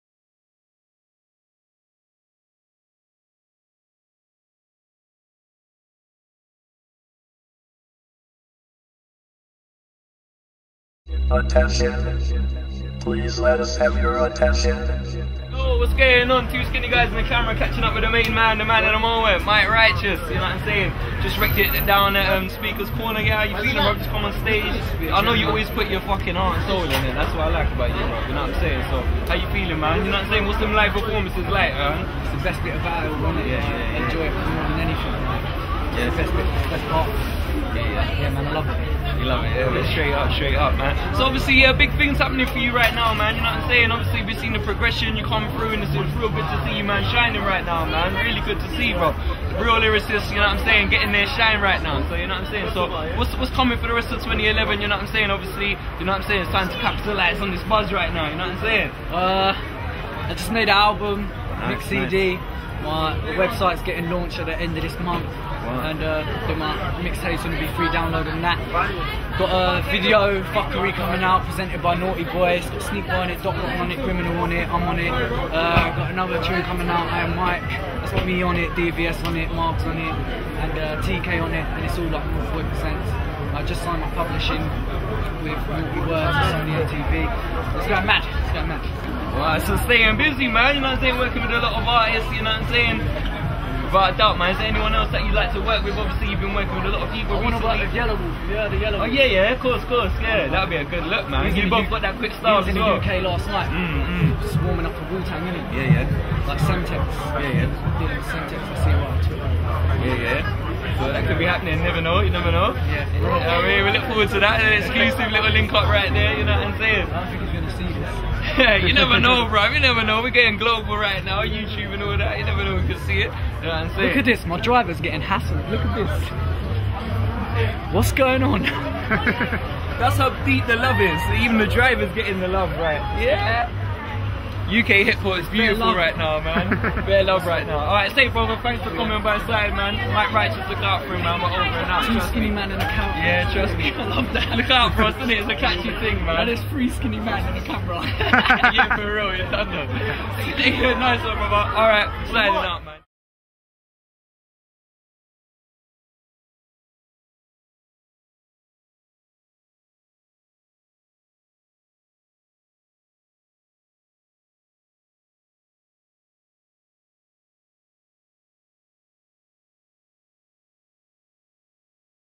Mic Righteous Talks New Publishing Deal
2 Skinny Guys And A Camera, AKA Mr 13 and DJ Bones, caught up with Mic Righteous outside Speakers Corner last night. Righteous talks about his brand new publishing deal as well as what is to come from him in the rest of 2011. He also talks about who he would like to collaborate with. Catch this and many more UK Hip Hop interviews @